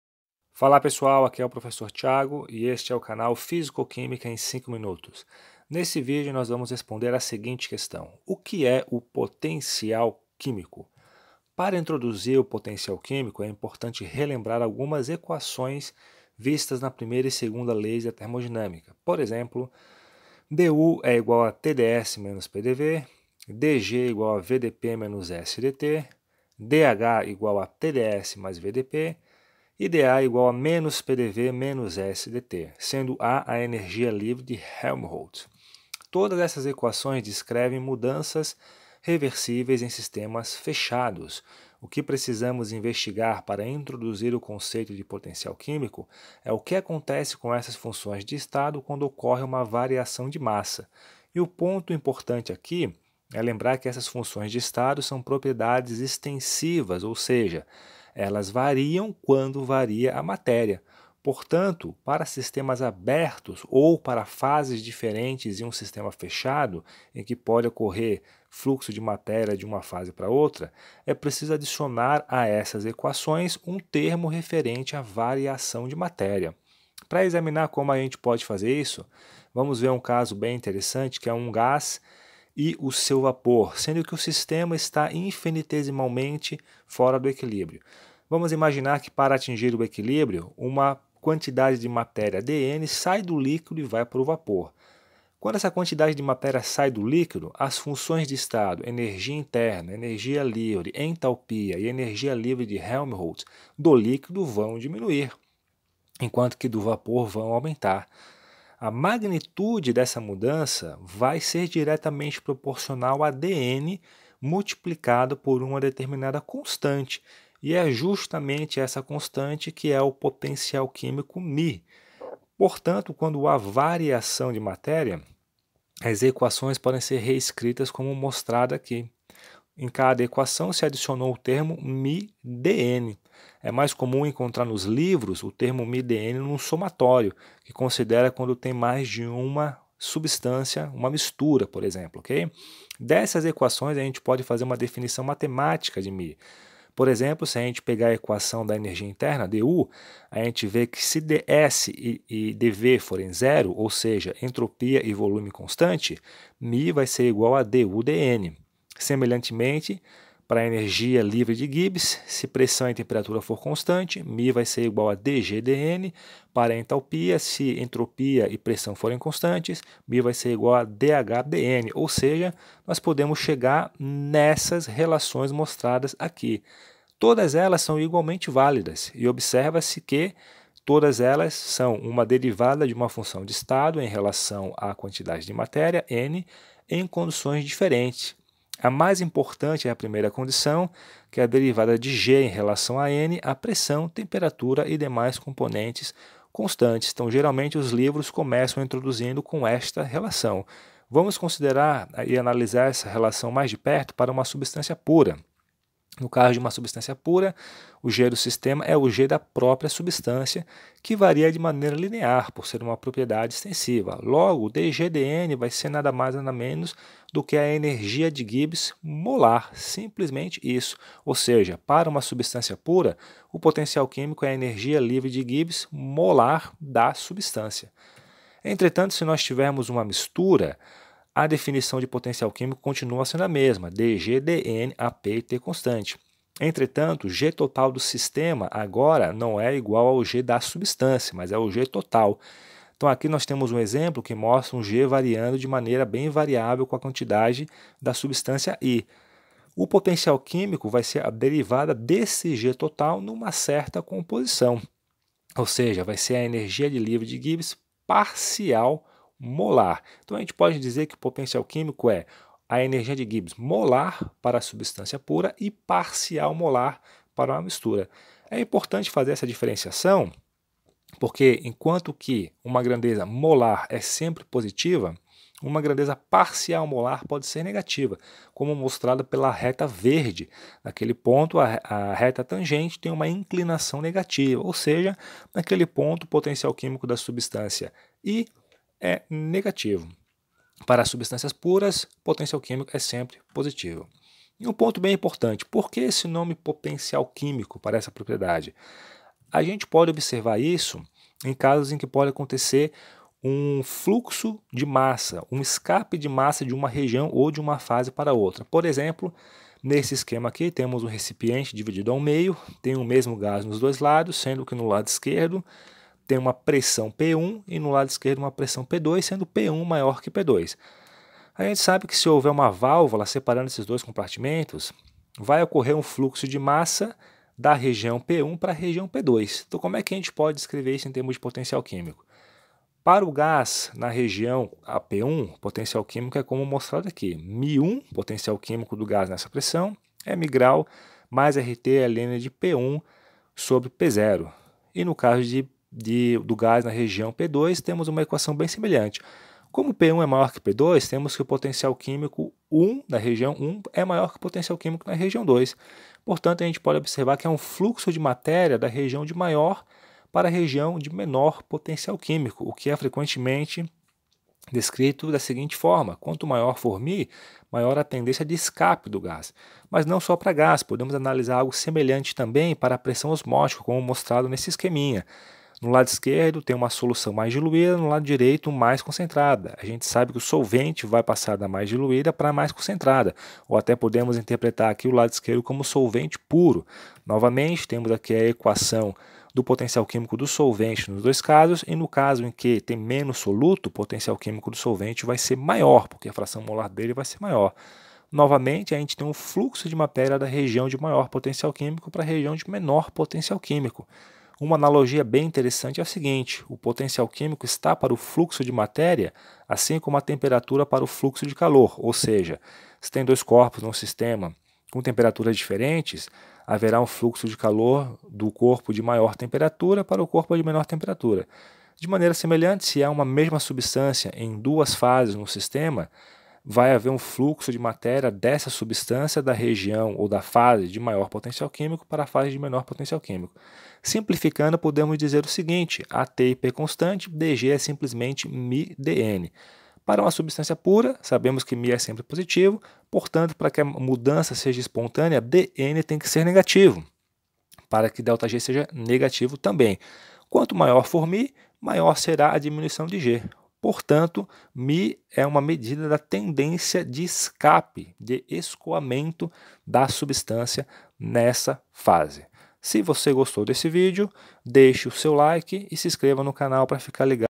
O que é potencial químico? (Fundamentos de termodinâmica)
Neste vídeo é apresentada a definição de potencial químico, um dos conceitos mais importantes em físico-química. São apresentadas as definições matemáticas, a interpretação do porquê desse nome e a utilização desse conceito para explicar o escoamento de gases de regiões de alta pressão para regiões de baixa pressão e do solvente de uma solução menos concentrada para uma solução mais concentrada (osmose).
A pressão e temperatura constantes, o potencial químico pode ser definido como a energia livre de Gibbs molar (para substâncias puras) e parcial molar (para misturas). Entretanto, é possível apresentar diferentes definições para o potencial químico em diferentes condições, como a volume constante, entropia constante, etc.